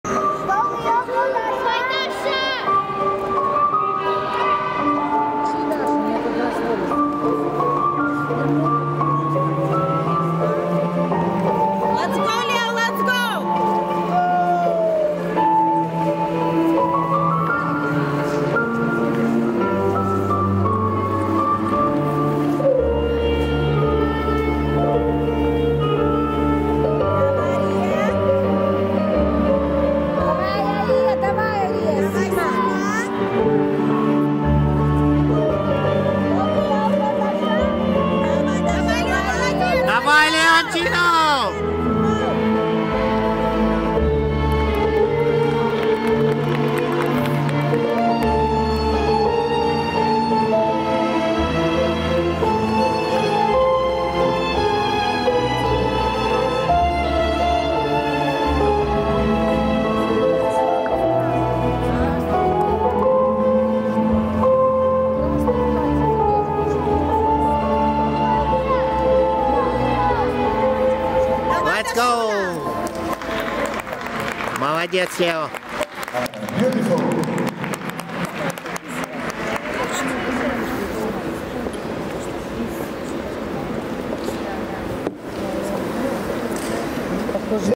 2019 Open Worlds U12 Juvenile Ballroom RS Semifinal Viennese Waltz
Leo Kolmykov and Daria Andreyev
Dublin, Ireland